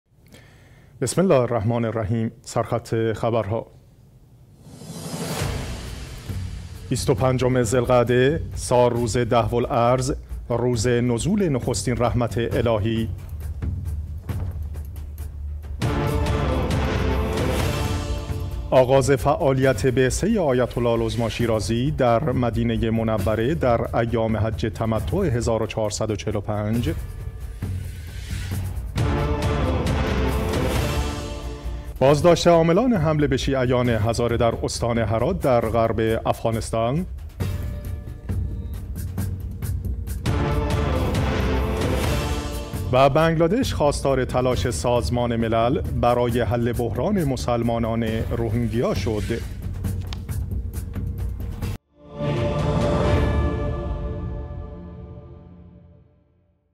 سرخط خبرها: یکشنبه 13 خرداد ماه 1403
۱ – ۲۵ ذی القعده؛ سالروز دحوالارض، روز نزول نخستین رحمت الهی

۲ - آغاز فعالیت بعثه آیت الله العظمی شیرازی در مدینه منوره در ایام حج تمتع ۱۴۴۵

۳ - بنگلادش خواستار تلاش سازمان ملل برای حل بحران مسلمانان روهینگیا شد

۴ - بازداشت عاملان حمله به شیعیان هزاره در استان هرات در غرب افغانستان
۵- آیت الله العظمی شیرازی: در دوران حکومت رسول خدا و امیرالمومنین صلوات الله علیهما هرگز زندان، شکنجه، ضرب و شتم و قتل مردم دیده نشده، و فقط در برخی از موارد، احیانا و صرفا از کلام تهدید استفاده شده است